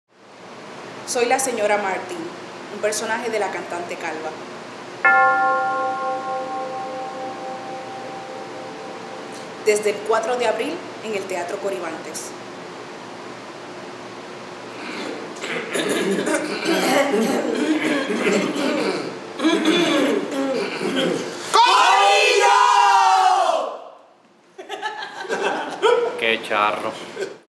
Lizmarie Quintana en La Cantante Calva
La Cantante Calva
de Eugene Ionesco
Dirige Emineh de Lourdes

Desde el 4 de abril
Teatro Coribantes
767-7400
Ticket Center
792-5000